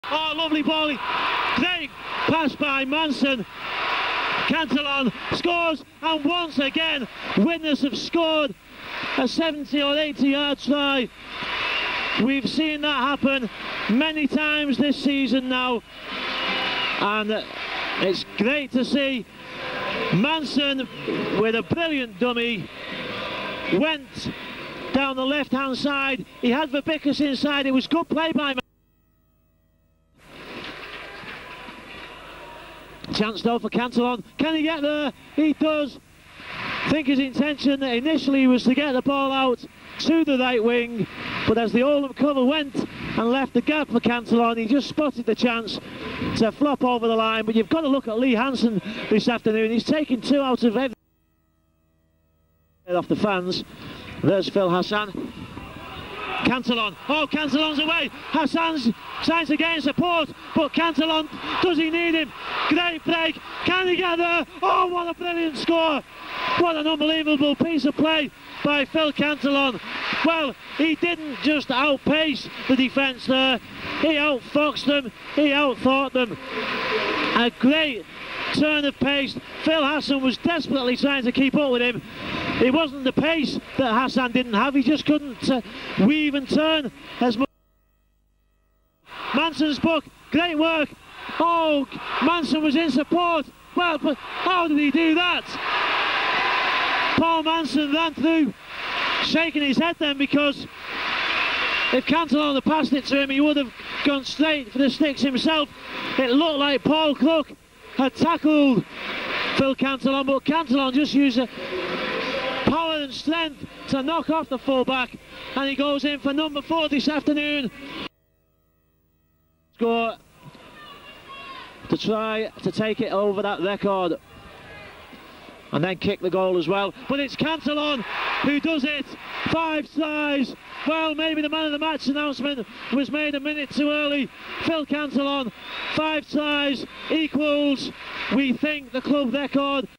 Phil Cantillon's - 5 tries in a match Widnes Vikings record
Cantillon breaks 5 tries in a match record in 1999 for Widnes Vikings